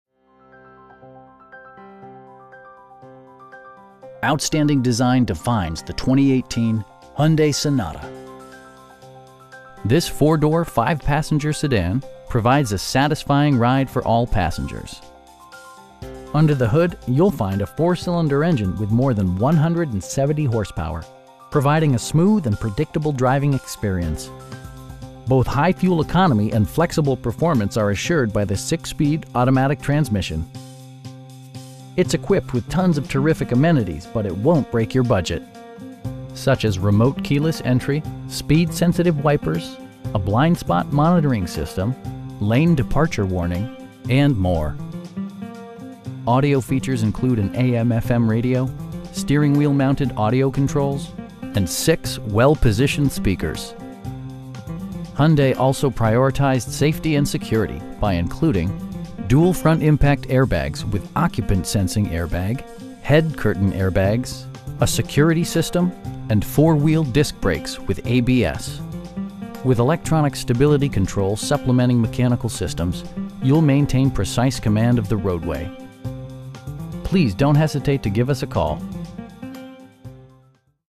2018 Hyundai Sonata SEL in Houston, TX 77074
Sterling McCall Hyundai
10505 Southwest Freeway

Outstanding design defines the 2018 Hyundai Sonata. This 4 door, 5 passenger sedan provides a satisfying ride for all passengers. Under the hood you'll find a 4 cylinder engine with more than 170 horsepower, providing a smooth and predictable driving experience. Both high fuel economy and flexible performance are assured by the 6 speed automatic transmission. It's equipped with tons of terrific amenities, but it won't break your budget. Such as remote keyless entry, speed sensitive wipers, a blind spot monitoring system, air conditioning, lane departure warning, power windows, cruise control, and more. Audio features include an AM/FM radio, steering wheel mounted audio controls, and 6 well positioned speakers. Hyundai also prioritized safety and security by including: dual front impact airbags with occupant sensing airbag, head curtain airbags, traction control, brake assist, a security system, and 4 wheel disc brakes with ABS. With electronic stability control supplementing mechanical systems, you'll maintain precise command of the roadway. We know that you have high expectations, and we enjoy the challenge of meeting and exceeding them! Please don't hesitate to give us a call.